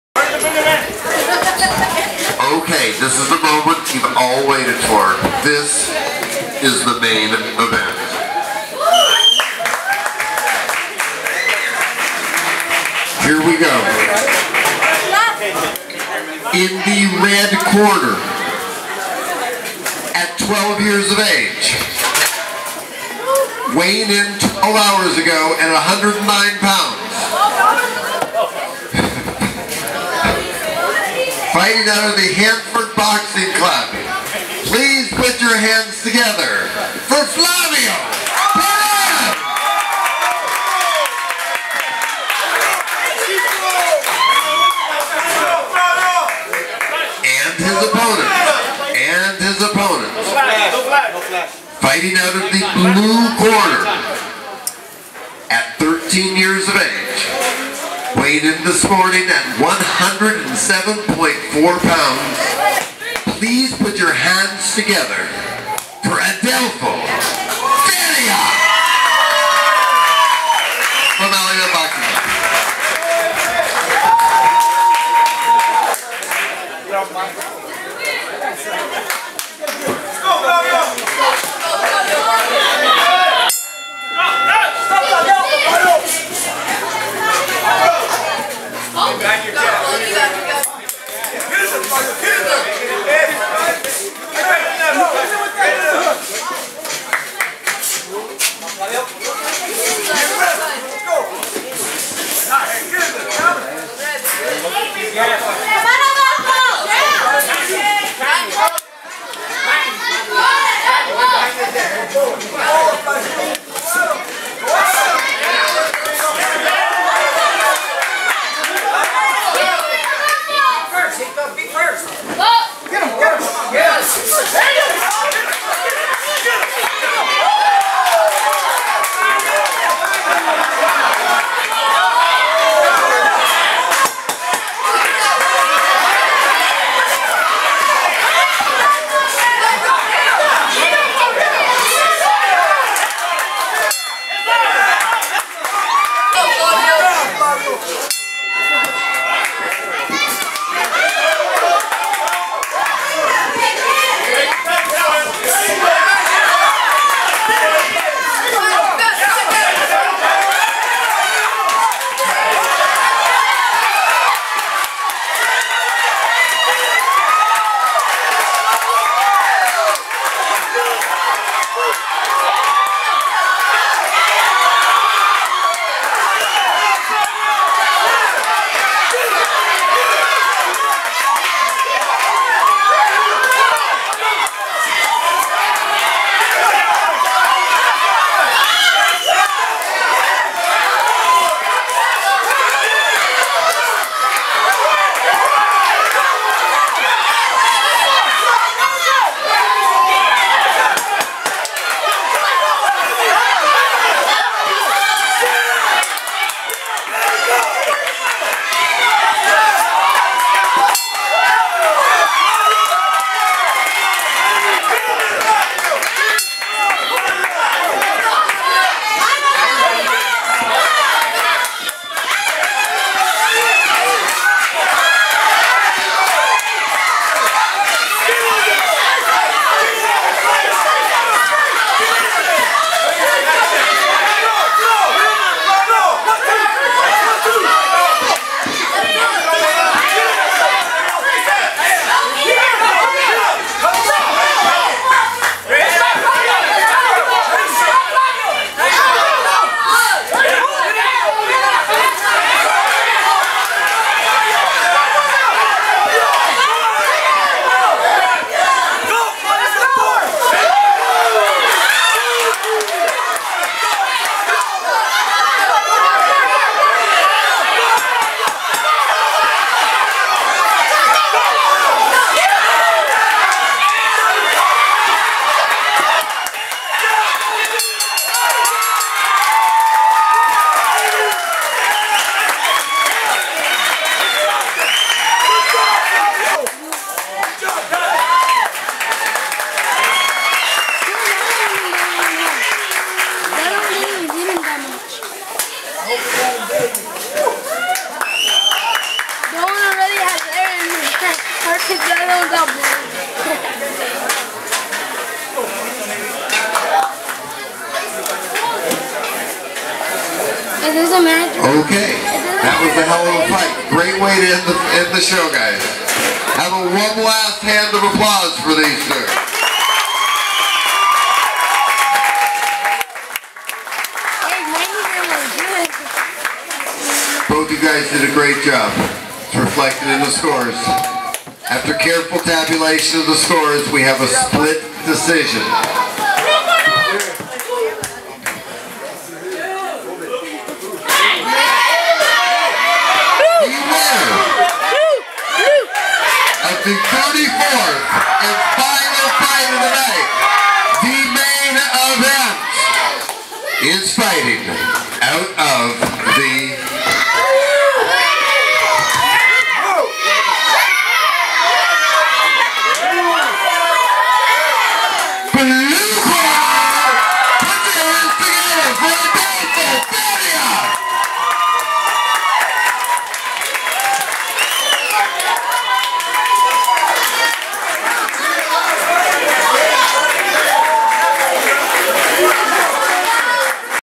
34 Flavio Perez -- Hanford BC 12 109 vs Adelfo Ferria -- Aleman Boxing Fresno 13 107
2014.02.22 Aleman Boxing Fresno Show

34 Flavio Perez -- Hanford BC 12 109 vs Adelfo Ferria -- Aleman Boxing Fresno 13 107

We had 33 bouts/7 hours of boxing this day

Aleman Boxing Fresno gets results!
The Best Boxing Gym in Fresno CA
On the corner of Olive / Sierra Vista

 CESAR CHAVEZ The Movie 4/4/14